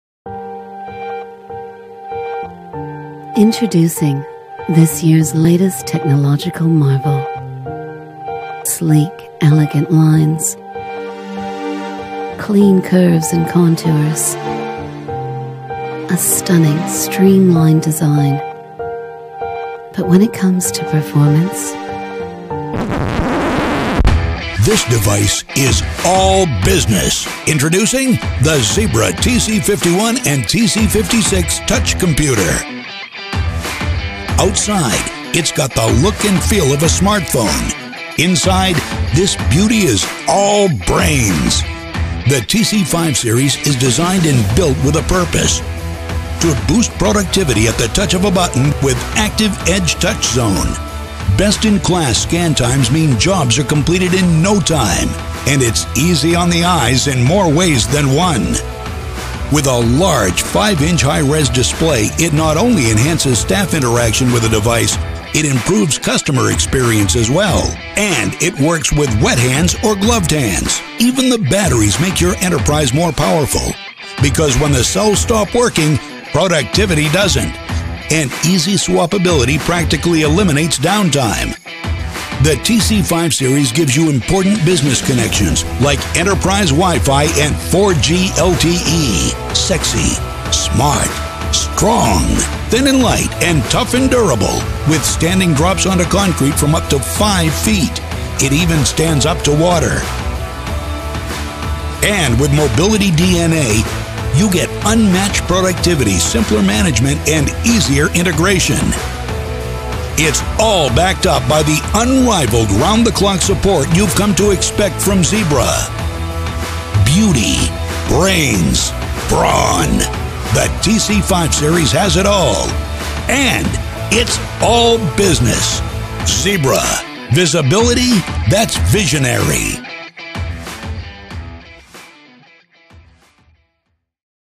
ZEBRA INTRODUCES SLEEK, SMART MOBILE COMPUTERS WITH BEAUTIFUL DESIGN, ENTERPRISE DURABILITY
The Zebra TC5 Series ( TC51 - TC56 ) is a modern and easy to use as consumer smartphones so there is no need to compromise device design for enterprise functionality.

The devices are built to last for years and designed to withstand accidental drops into water or onto concrete and used in dusty areas - providing enterprise-class durability.

Running the Android 6.0 (Marshmallow) operating system, the TC5 Series is supported by Zebra’s Mobility DNA, the industry’s most comprehensive suite of application security, development tools and mobile end-user apps.

With a 1.8 GHz hexa-core 64-bit processor, the TC5 series helps enterprise apps run up to five times faster while consuming 15 percent less power than other devices running on quad core platforms.

The TC5 Series features innovative productivity design features to help improve user satisfaction including an easily removable and replaceable battery pack, a large 5-inch display and customizable Active Edge™ Touch Zones for one-touch access to the most frequently used device features and apps.

The TC5 series also provides the operational ease of support for a better total cost of ownership (TCO) as well as the fastest, most dependable wireless connections inside and outside with 802.11ac/r/k Wi-Fi connectivity for superior roaming capability, and 4G LTE WWAN.